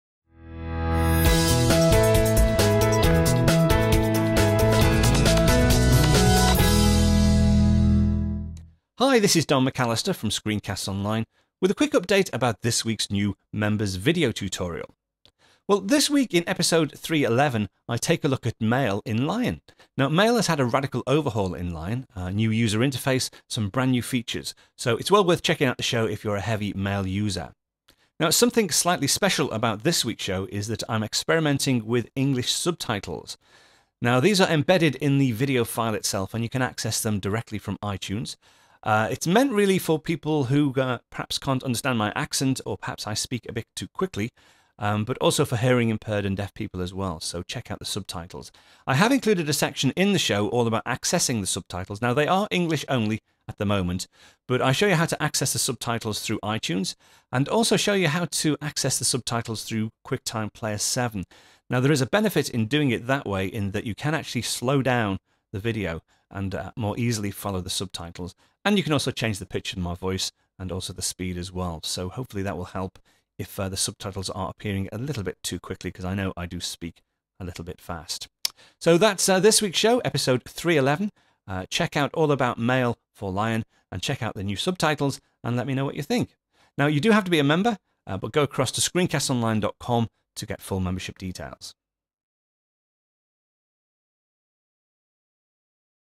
SCO0311 - Mail in Lion Trailer (with Subtitles)
A brief trailer for a ScreenCastsOnline members show with full embedded English subtitles